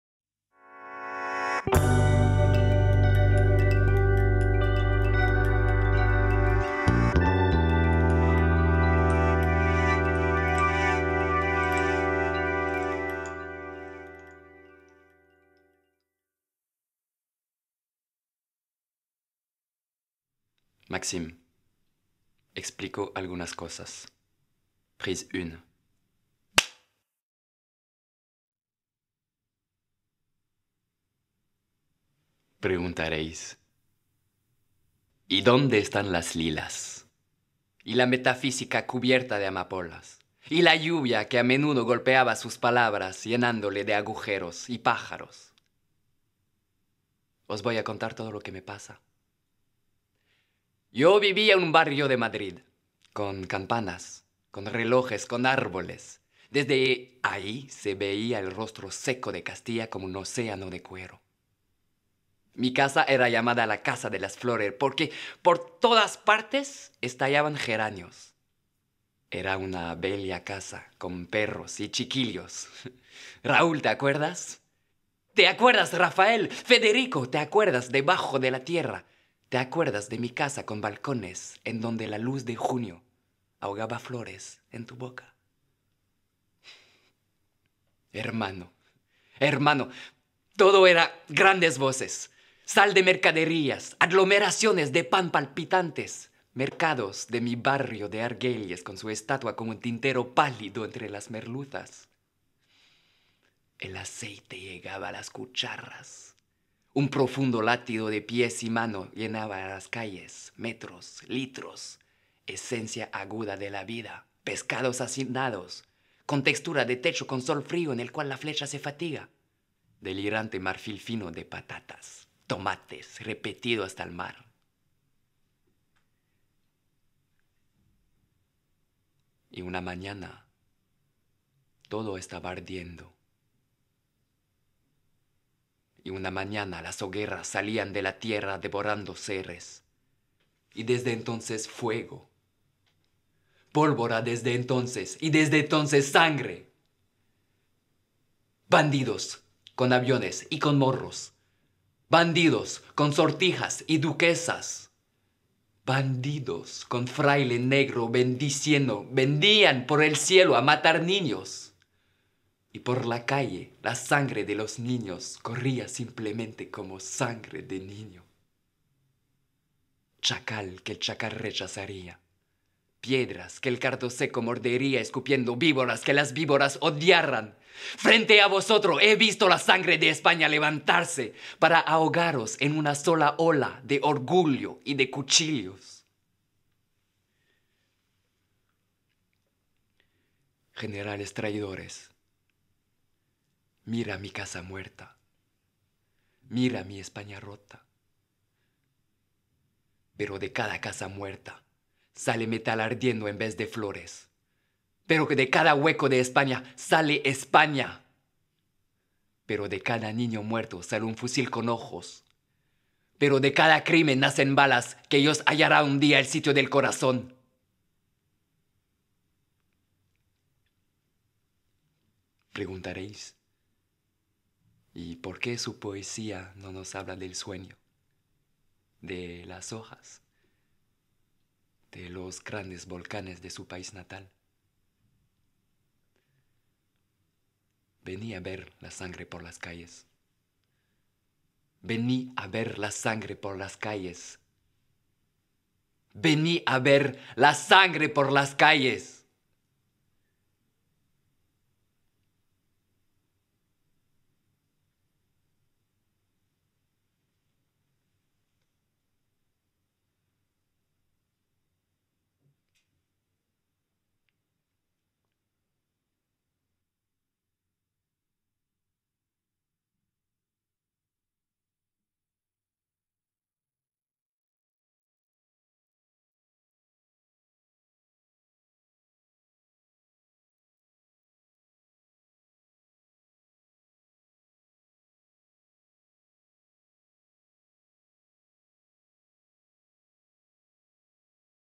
Explico algunas cosas, Pablo Neruda | PoAime
Pour ce neuvième épisode de PoAime,
Maxime Mannevy nous interprète un magnifique poème en espagnol, Explico algunas cosas de Pablo Neruda.